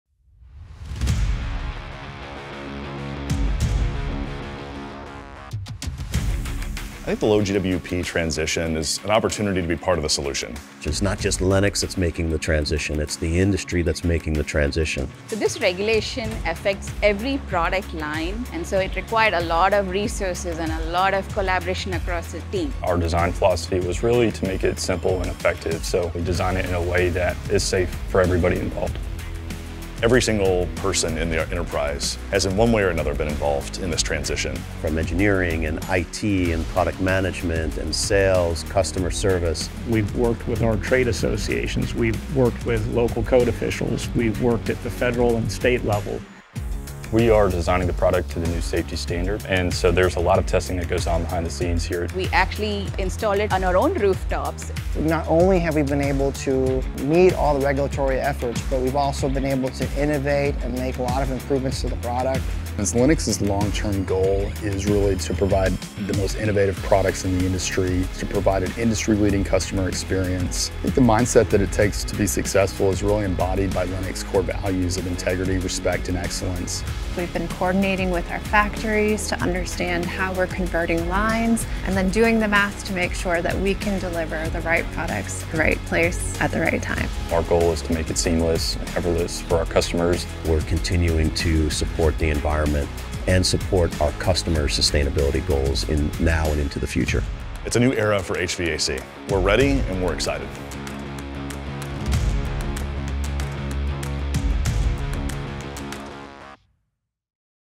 Lennox Commercial | Supporting Your Seamless Shift to Low GWP Systems
Lennox is helping you prepare for HVAC projects in 2025 and beyond with everything your team needs to stay informed and competitive.

Across North America, the HVAC industry is converting to refrigerants with lower global warming potential. Our newly enhanced product lines are precision engineered for Low GWP refrigerants—and safe and reliable as ever. Plus, we’re helping you navigate the road ahead with access to expert training, expanded online resources, and comprehensive support at every step, so you can convert with confidence.